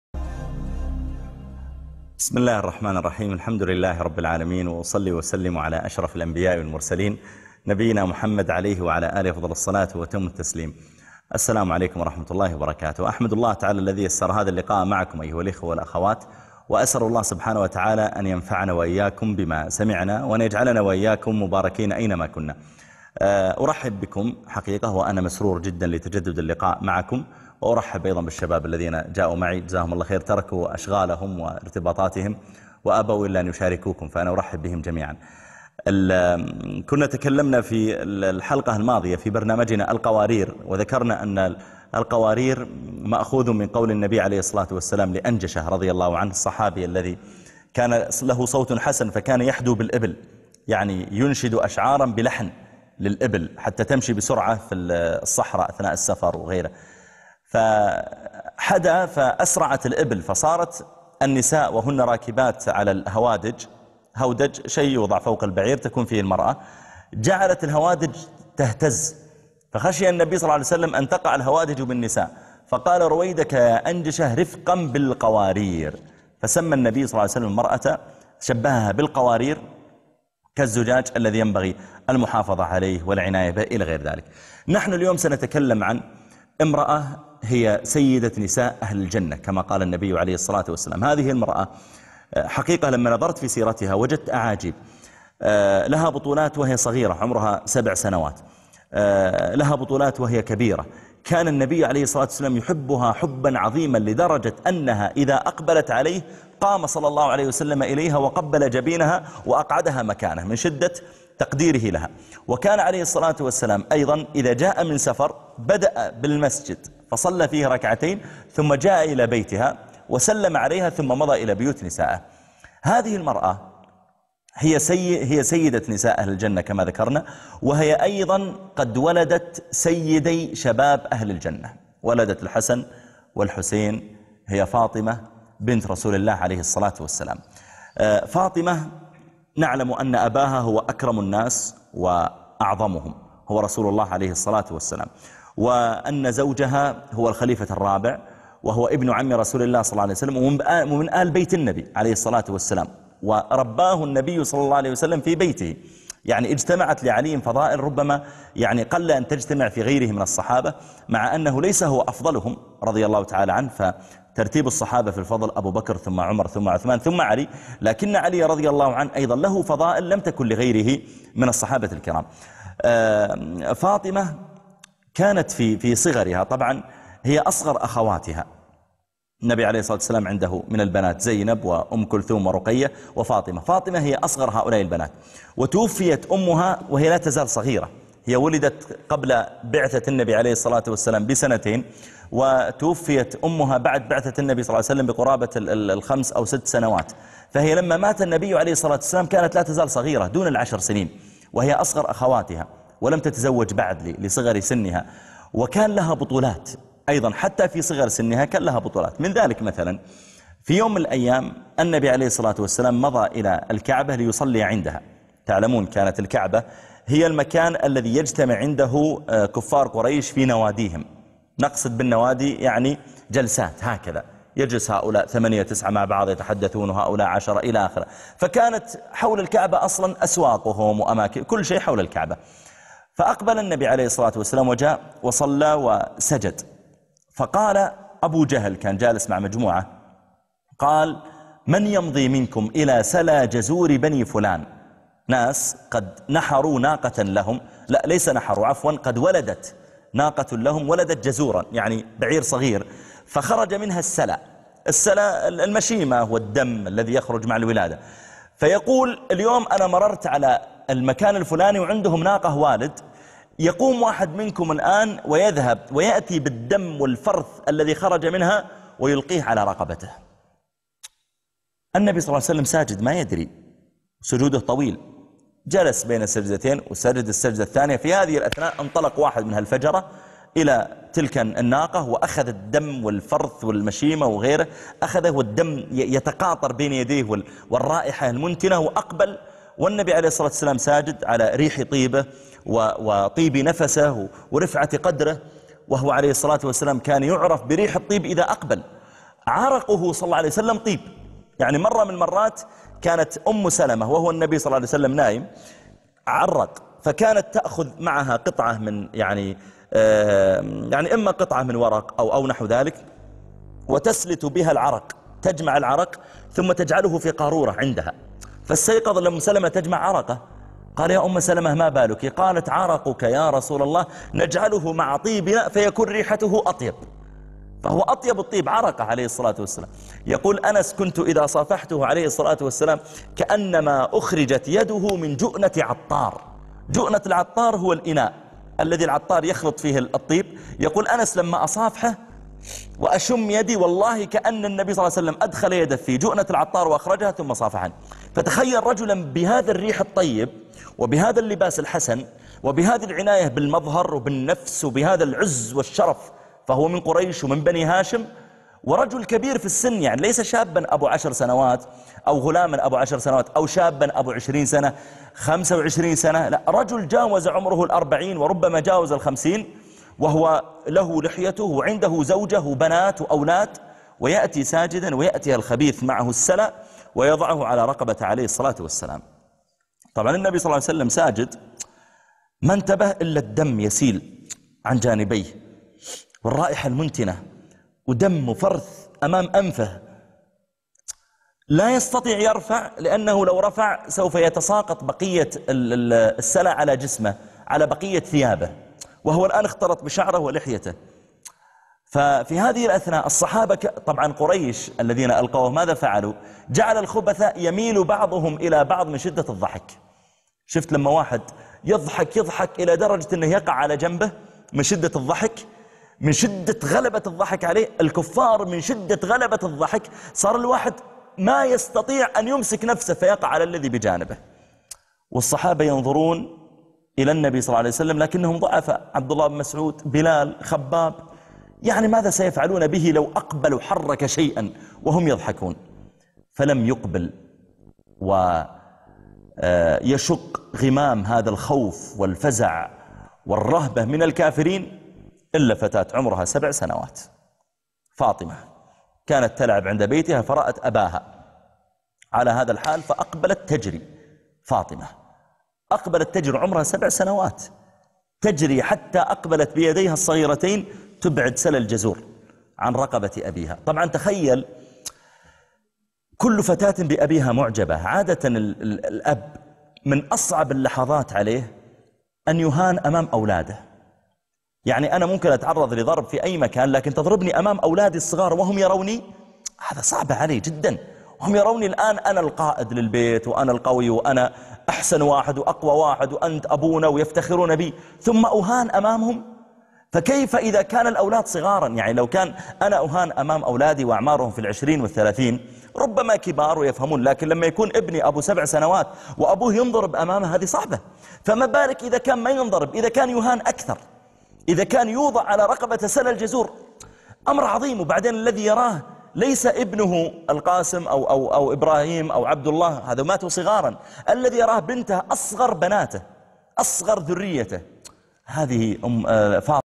The Virtues of Fatimah (Part 1)
Lecture by Sheikh Muhammad Al-Areefi on the life and virtues of the daughter of the Prophet Muhammad (peace be upon him), Fatimah Al-Zahraa'.

Lecture discusses the marriage of Fatimah to Ali bin Abi Talib, her relationship with Aisha (wife of the Prophet), and her bravery in defending the Prophet of Allah.

Miswar ibn Makhramah reported Allah's Messenger (may peace be upon him) as saying: Fatima is a part of me. He in fact tortures me who tortures her. (Sahih Muslim)

'A'isha reported that Allah's Messenger (may peace be upon him) called his daughter Fatima (during his last illness). He said to her something secretly and she wept. He again said to her something secretly and she laughed. 'A'isha further reported that she said to Fatima: What is that which Allah's Messenger (may peace be upon him) said to you secretly and you wept and then said to you something secretly and you laughed? Thereupon she said: He informed me secretly of his death and so I wept. He then again informed me secretly that I would be the first amongst the members of his family to follow him and so I laughed. (Sahih Muslim)

'Abdullah bin 'Abbas narrated: "The Prophet Muhammad (sal Allahu alaihi wa sallam) drew four lines and said to the Companions, "Do you know what these are?" They said, "Allah and His Messenger know best." He said, "The best women of the women of Paradise are Khadeejah bint Khuwaylid, Fatimah bint Muhammad, Maryam bint Imran, and Aasiyah bint Muzahim (the wife of the Pharaoh)." (Ahmad)